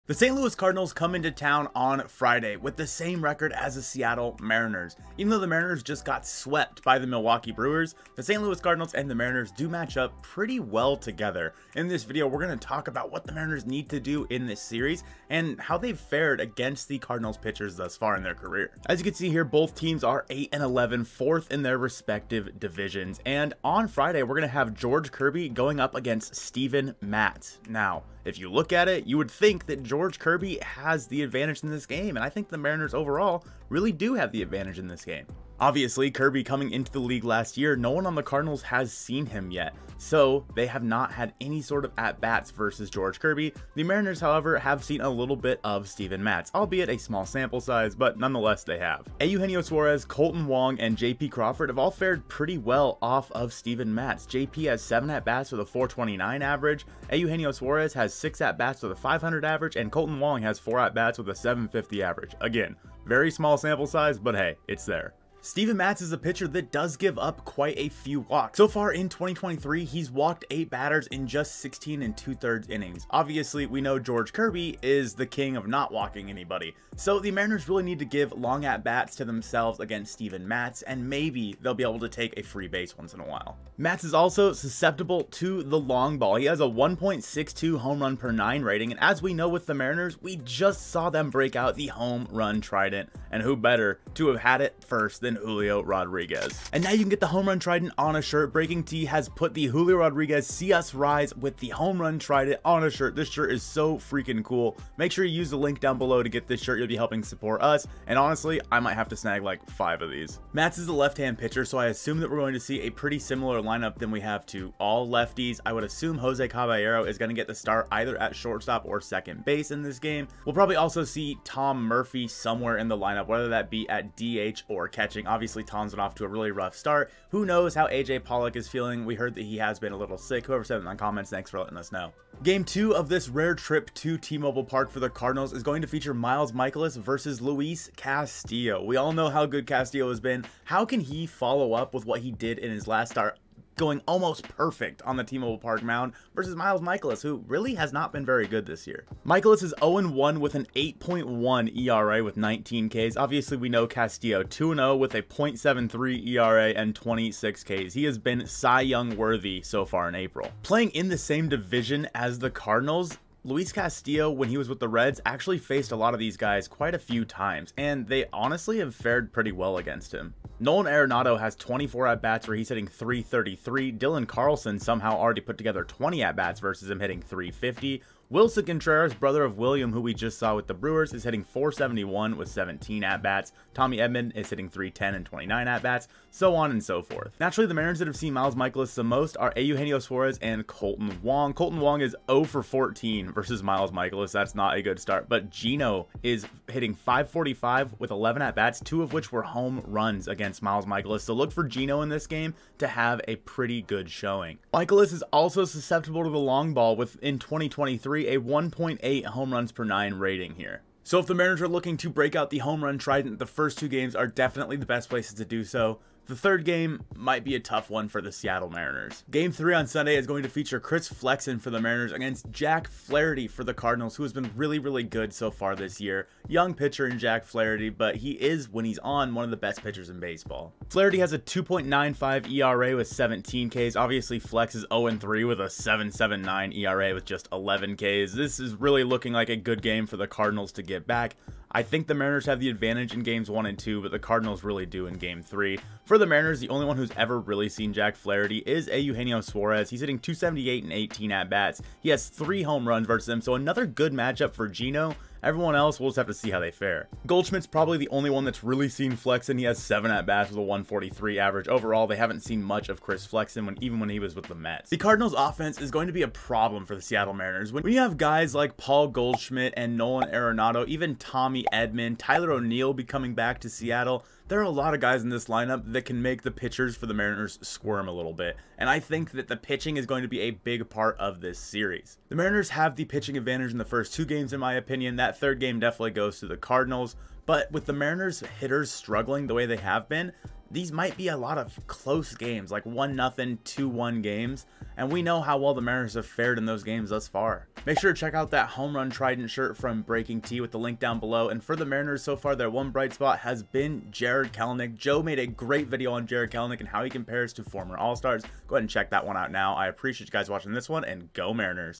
Mariners On the Verge of Turning it Around Against Cardinals?
Mariners On the Verge of Turning it Around Against Cardinals? The Mariners were just swept by the Brewers and now welcome the Cardinals to T-Mobile Park. This video is a preview of the Mariners-Cardinals series and what the Mariners need to do vs the Red Birds to right the ship.

🎥Behind the scenes videos from Mariner Mojo!

👕BreakingT to head straight to the Mariners specific merchandise!